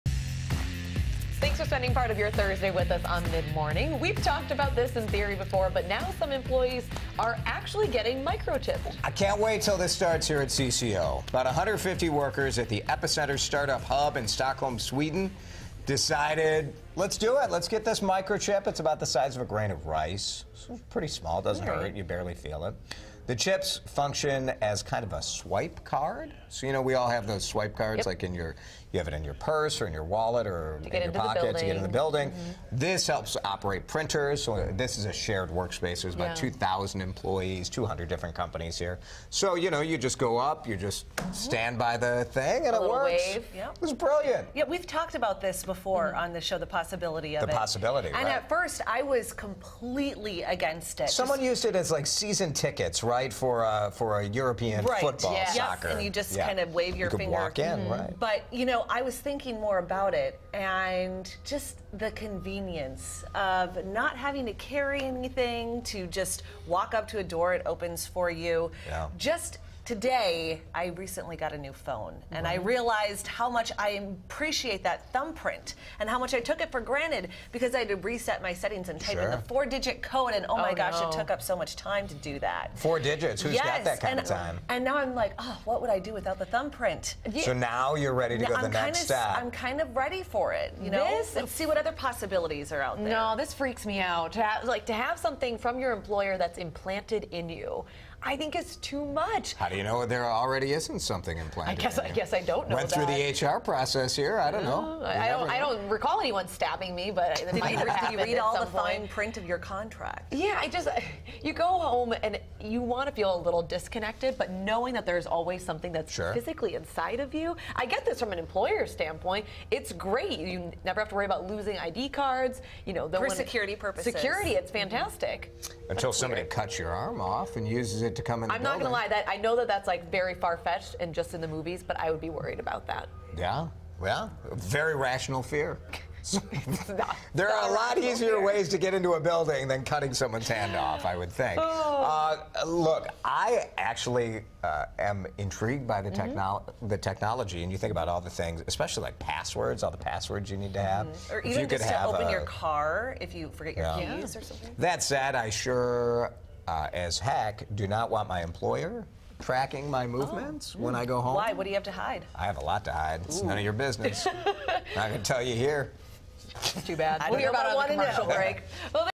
Panel Discussion: Employee Microchips
About 150 workers at the 'Epicenter' start up hub in Stockholm, Sweden ave decided to get a microchip the size of a grain of rice implanted. The news team discusses. (2:49)
WCCO Mid-Morning – April 4, 2017